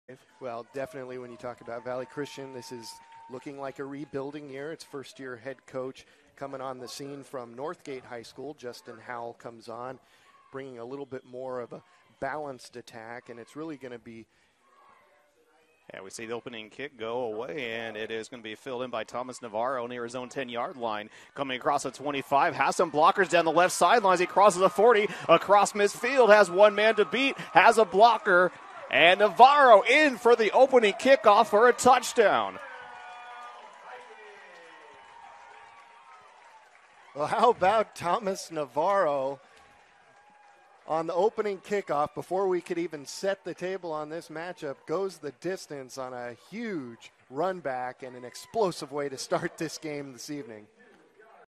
VC - #4 Navarro - Kickoff TD Return
Start of game kickoff return for touchdown